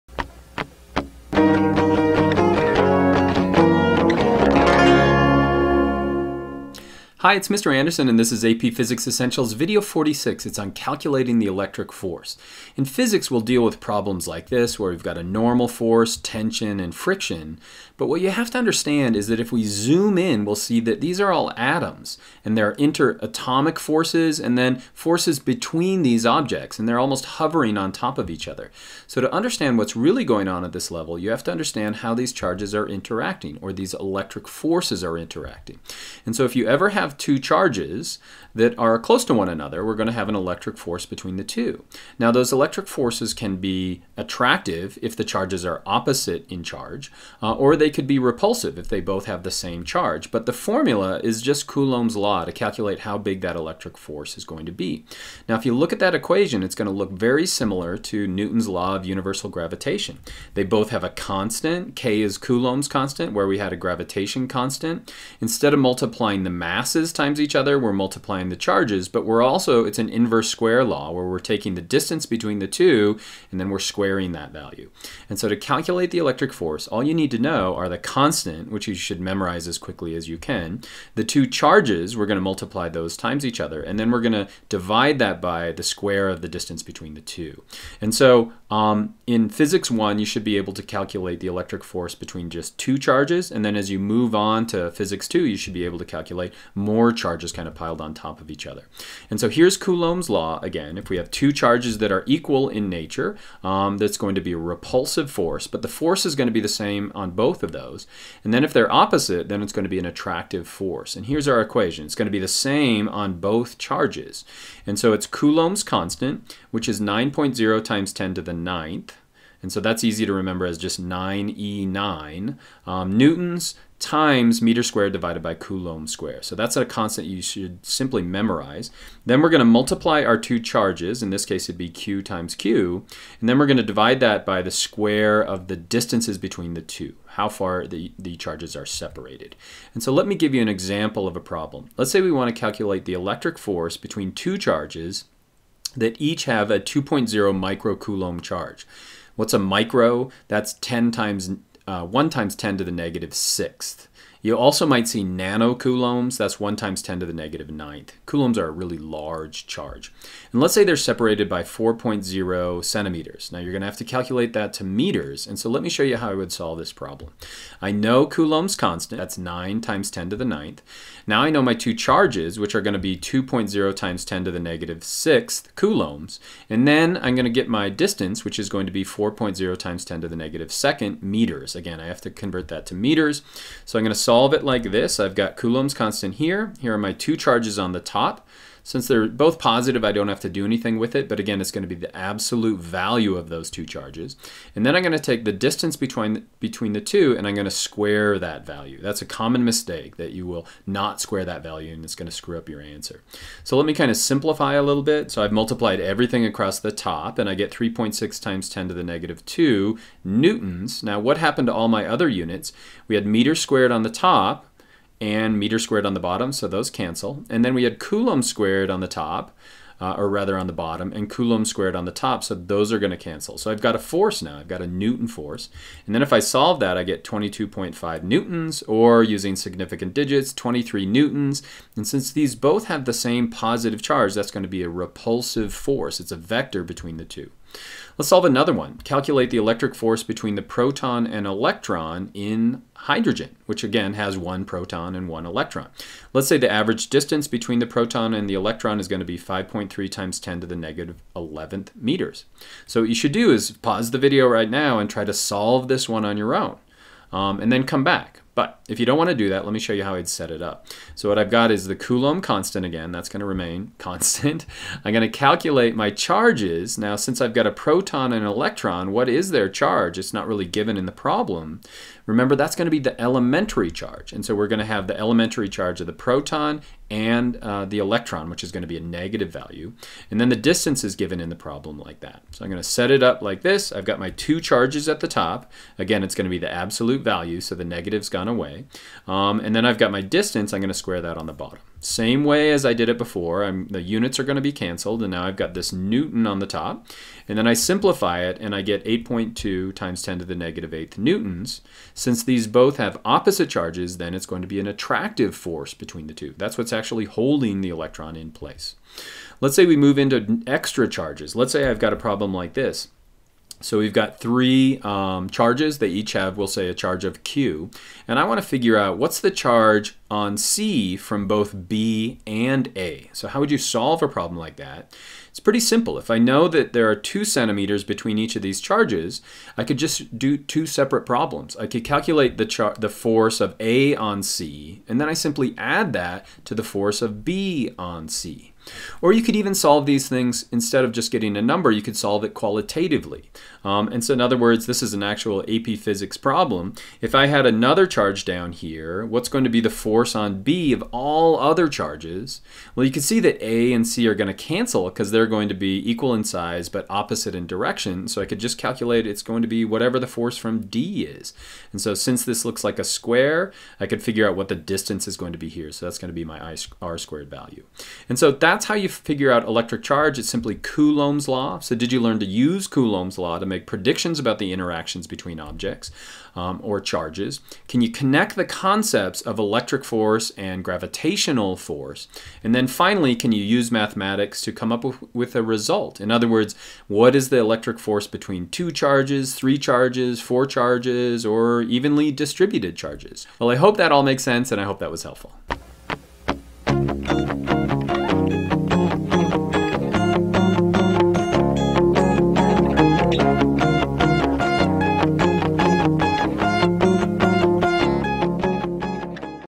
Calculating the Electric Force
046 - Calculating the Electric Force

In this video Paul Andersen explains how you can use Coulomb's Law to determine the electric force between two charges.  In Physics 1 students should be able to calculate the force between two charges and in Physics 2 students should be able to determine the force between up to four different charges.

“Coulomb’s Law.” Wikipedia, the Free Encyclopedia, August 17, 2014.
Mills, Ben. Ball-and-Stick Model of Part of the Crystal Structure of Cellulose Iβ., April 24, 2009. Own work.